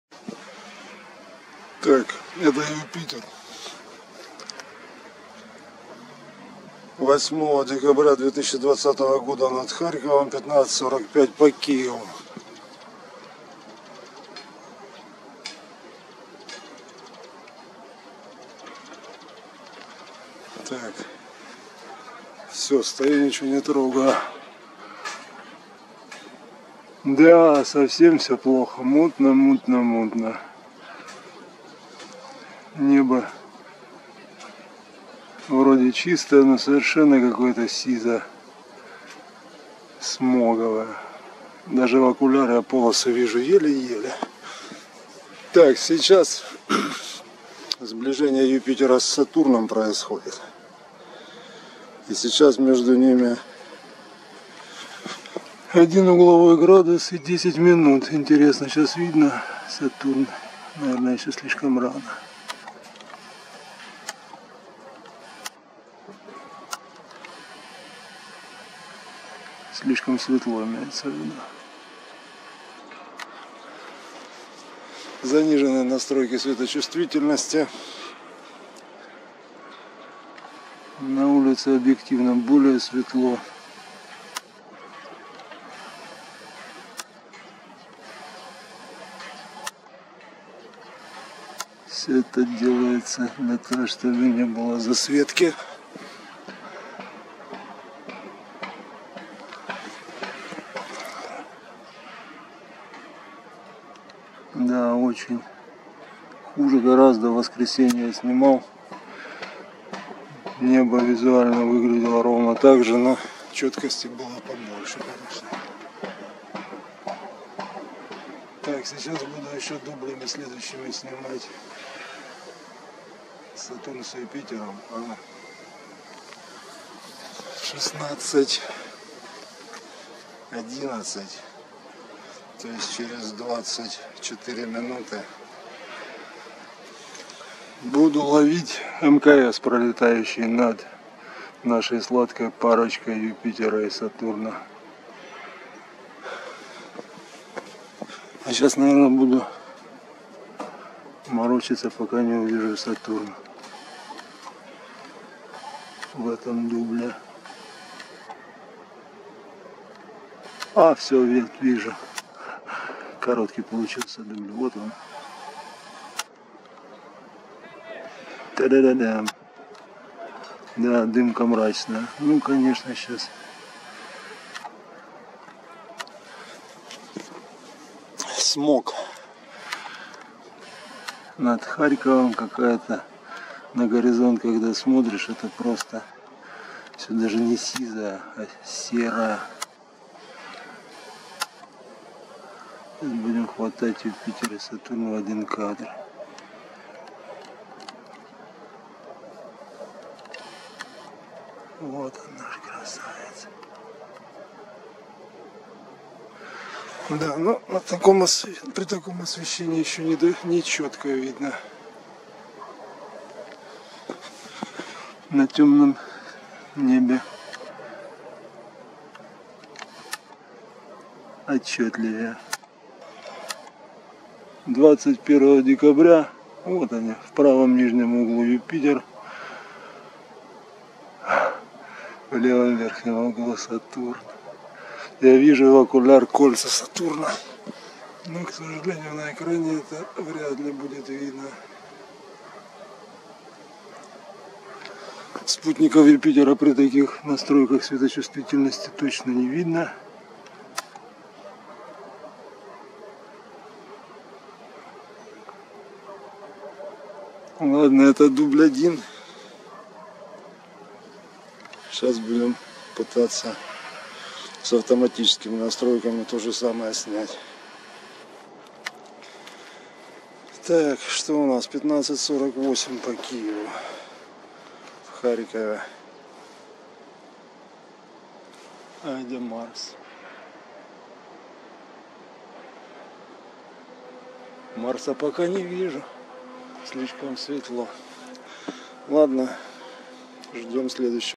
Юпитер и Сатурн - сближение на вечер 8 декабря 2020 года
Юпитер и Сатурн сближаются на небосводе - видео снято после заката 8 декабря 2020 года в Харькове.  Расстояние между Юпитером и Сатурном один угловой градус и десять минут. Максимальное сближение произойдёт 21 декабря 2020.